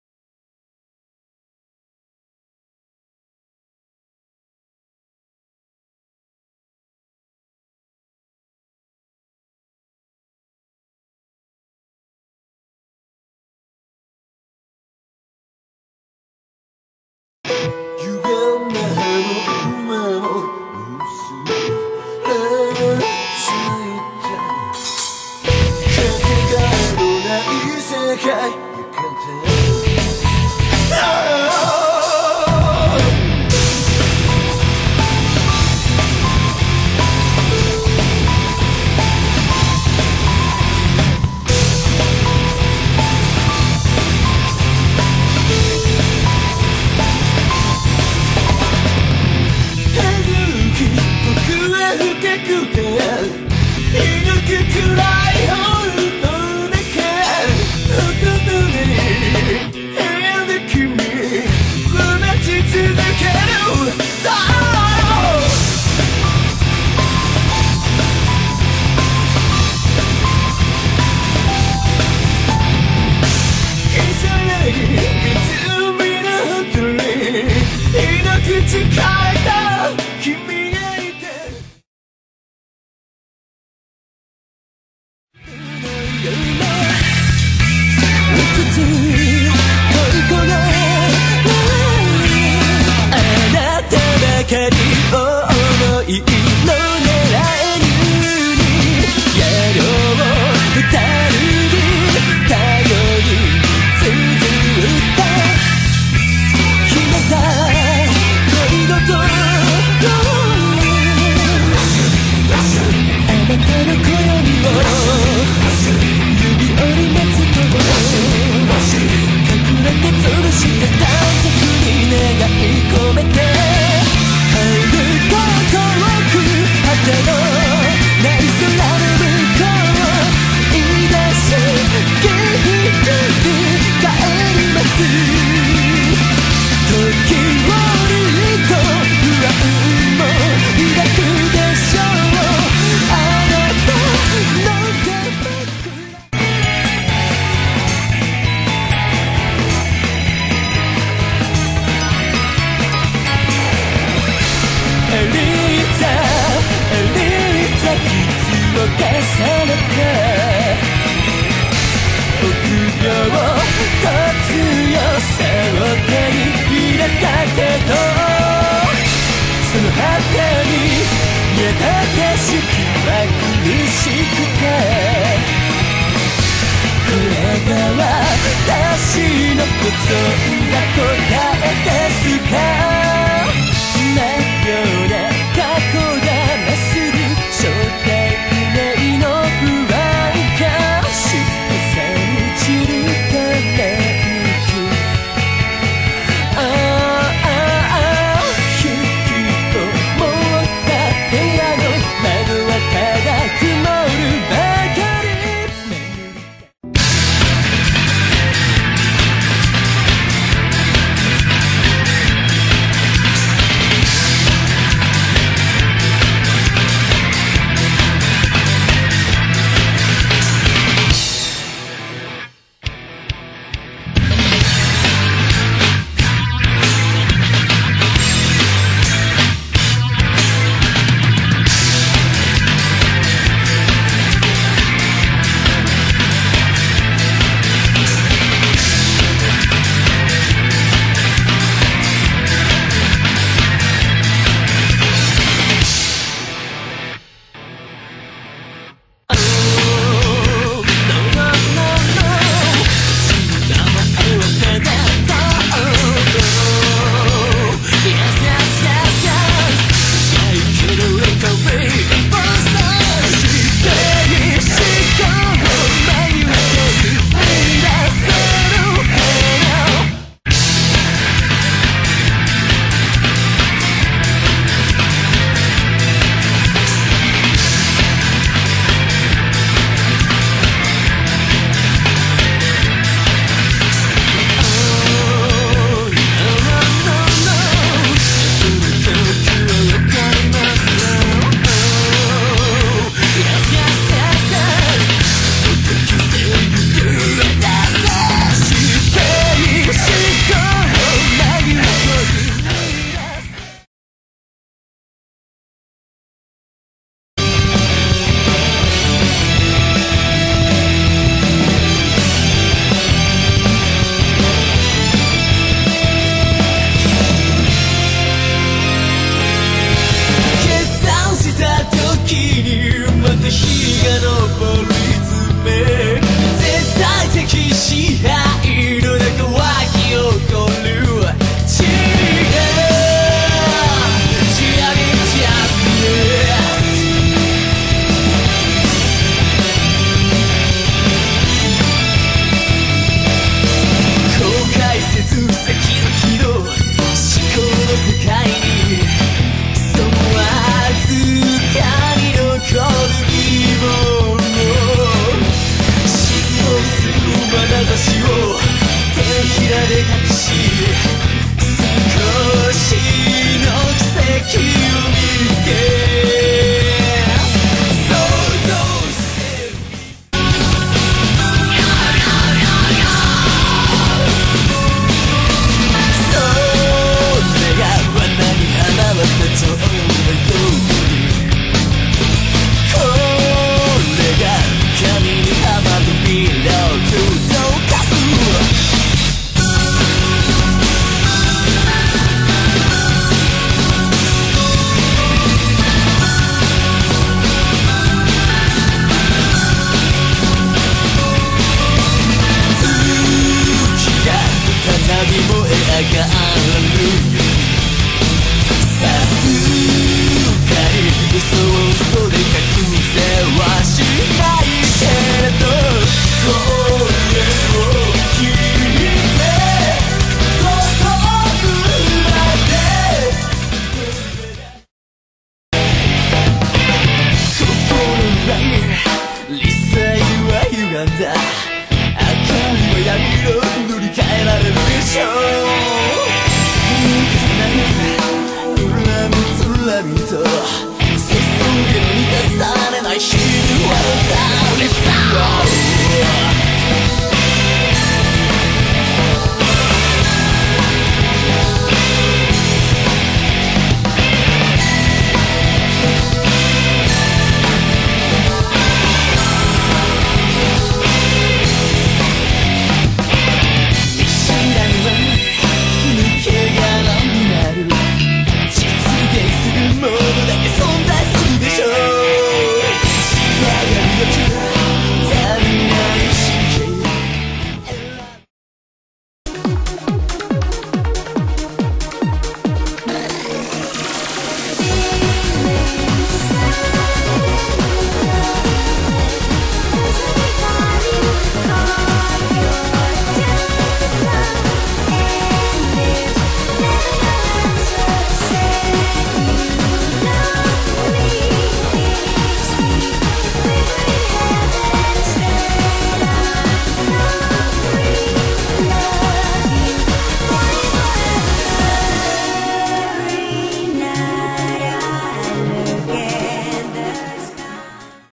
Undercode Acceleration
Compliation of three of Undercode's bands. Vidoll, 12012, and Phantasmagoria.